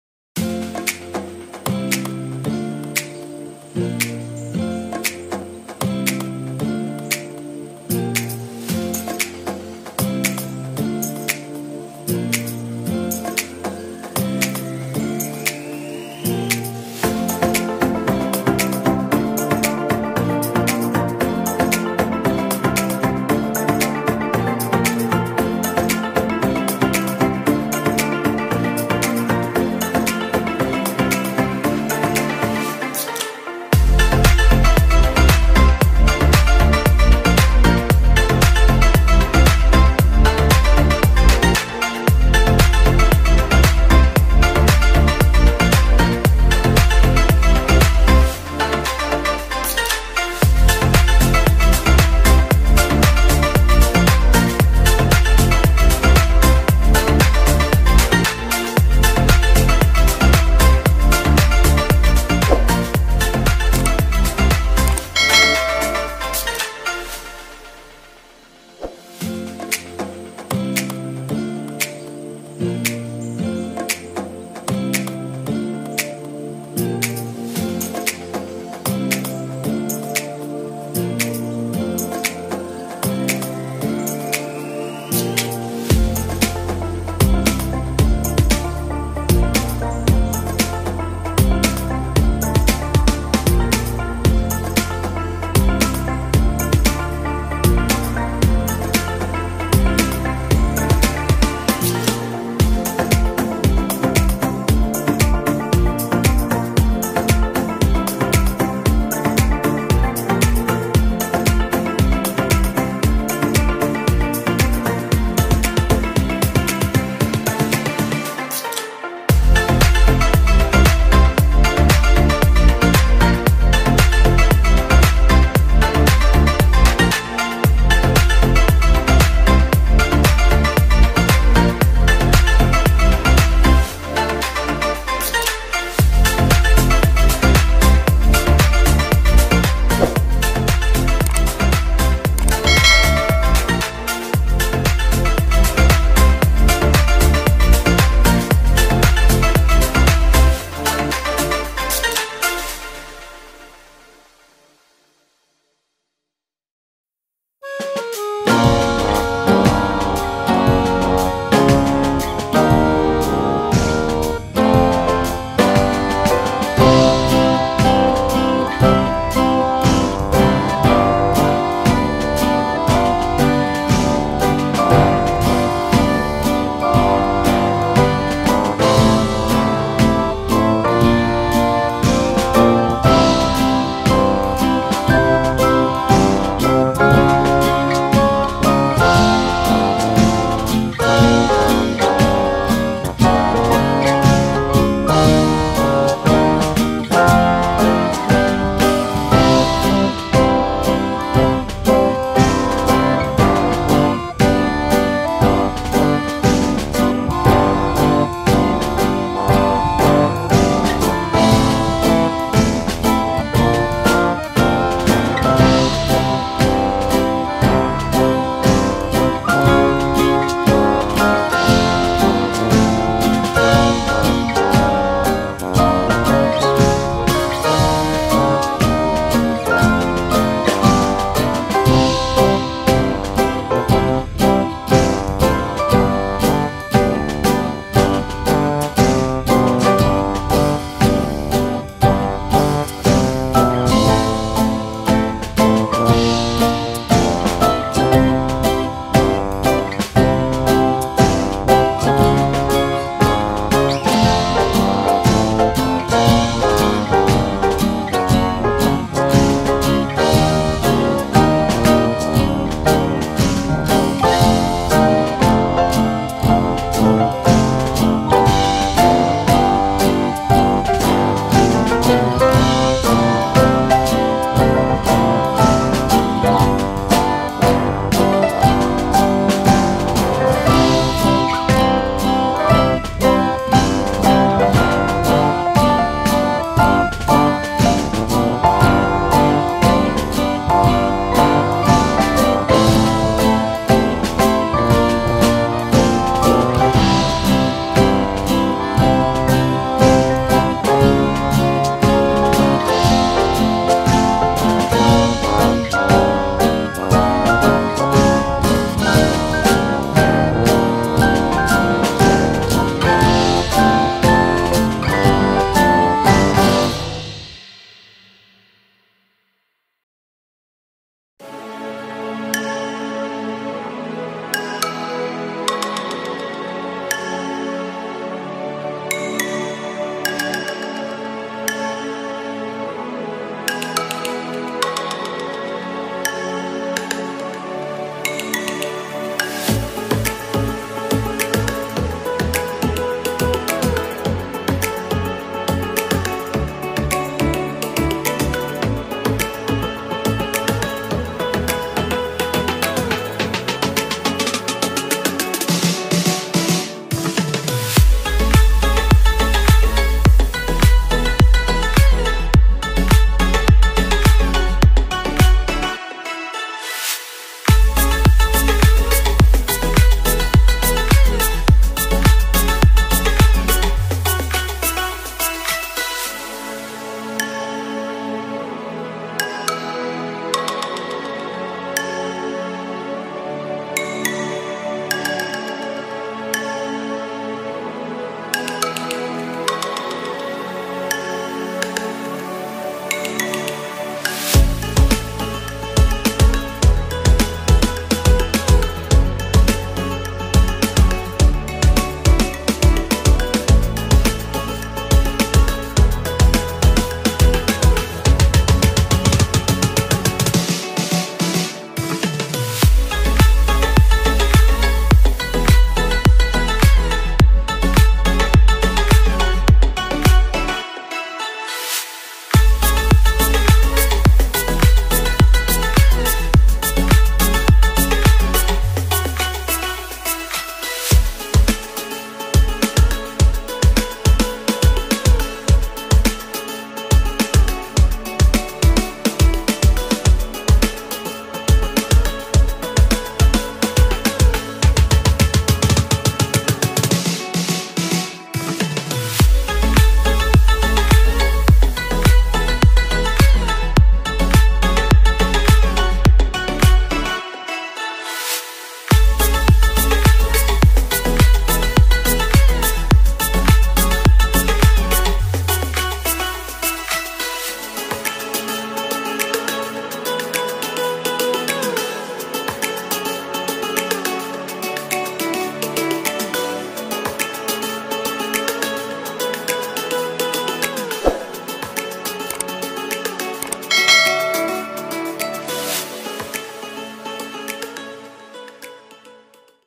So Creative Chocolate Cake Decorating ideas | Yummy Cake Decorating Recipe
Hi everyone. this is Simple way to make this cake and decorating tutorial as well you guys just used this step and make a cake for your family and friends any occasion like birthday party . we share everyday some delicious, yummy and tasty recipe to  you can used this step and make delicious dessert at your home.

Thanksyou so much for watching.

  📽  watching  more video here:---